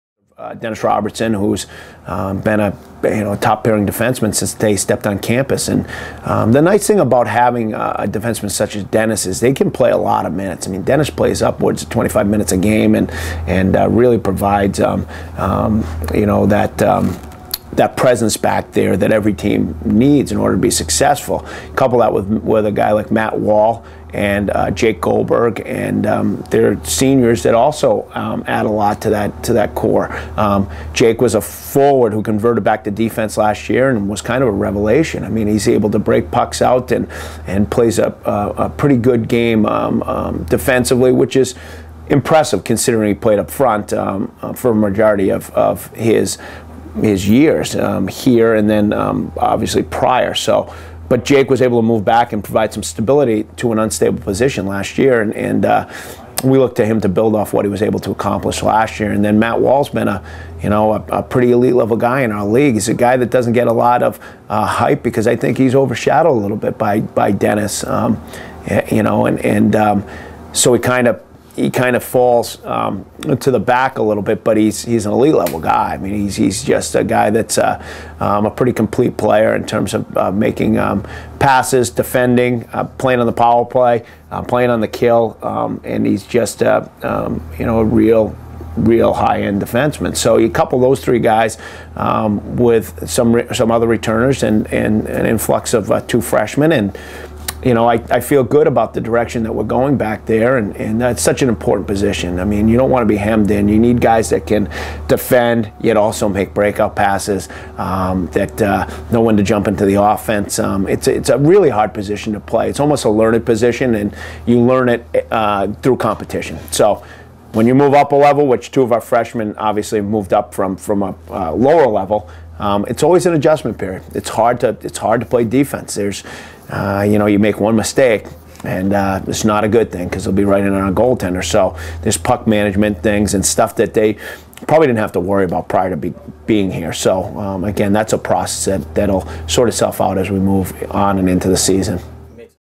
2013-14 Brown Men's Hockey Season Preview - Defense
In part three of the Brown Men's Hockey season preview, head coach Brendan Whittet takes a look at the defense heading into the 2013-14 campaign.